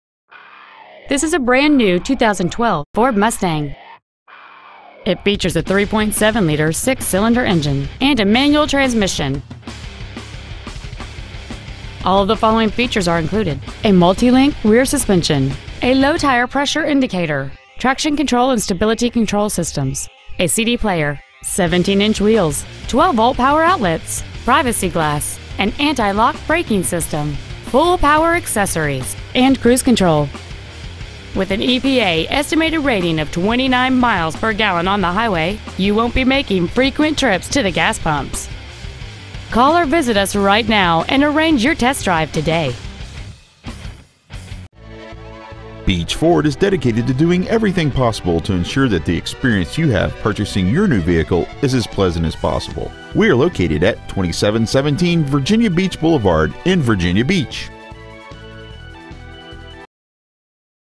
2012 Ford Mustang Virginia Beach VA
Year : 2012
 Make : Ford
 Model : Mustang
 Engine : 3.7L V6 24V MPFI DOHC
 Trans . : MANUAL
 Exterior : RACE RED
 Miles : 29

 2012 Ford Mustang Virginia Beach VA